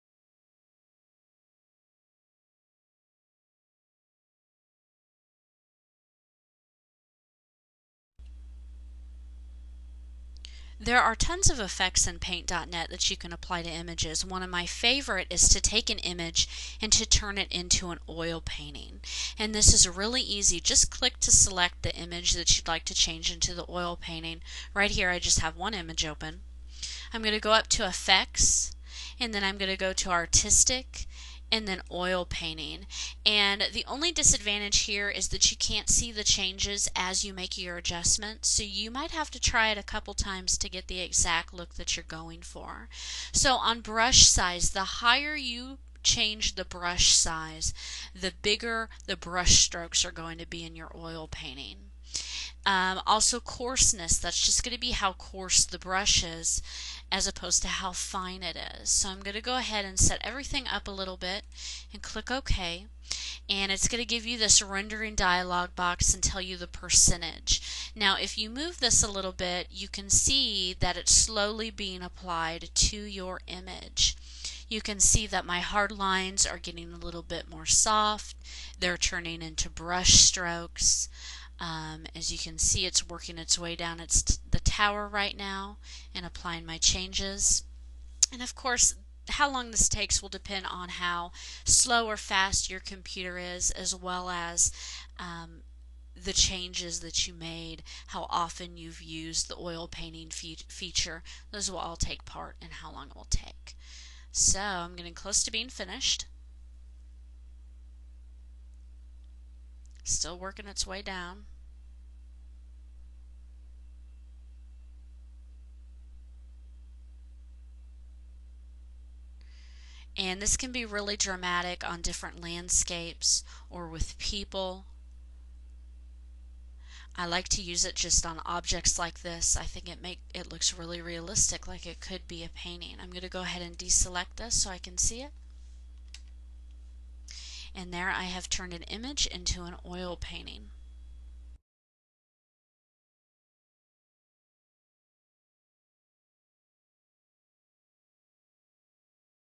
Create an Oil Painting From an Image in Paint.NET
See a higher quality video on TechTutor.TV! If you have a photo you would like to add the effect of it being painted, you can use the Create an Oil Painting effect in Paint.NET. In this video tutorial, you will learn how to create an oil painting from an image in Paint.NET. See higher quality tutorials all for FREE